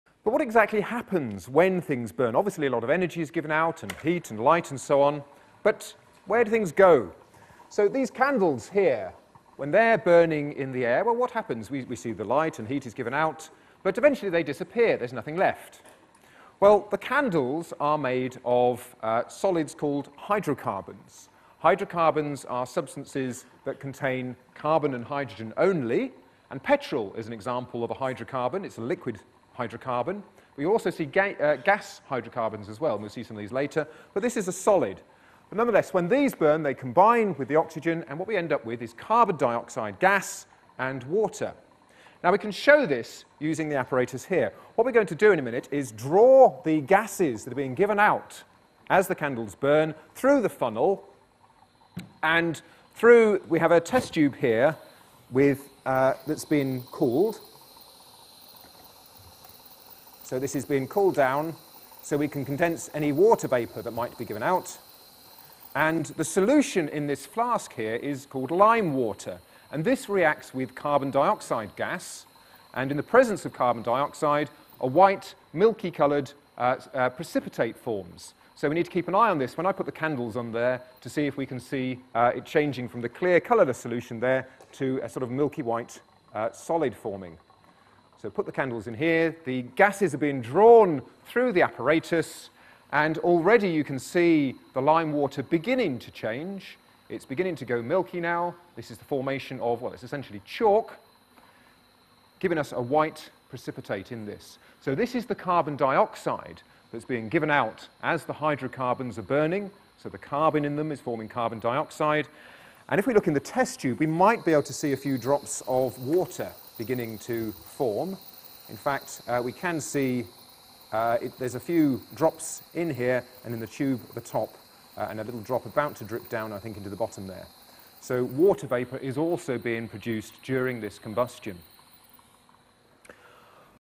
Fire and Flame 09 - Hydrocarbon Combustion Products
What is being made when hydrocarbons are burnt?

From the Peter Wothers lecture series - Fire and Flame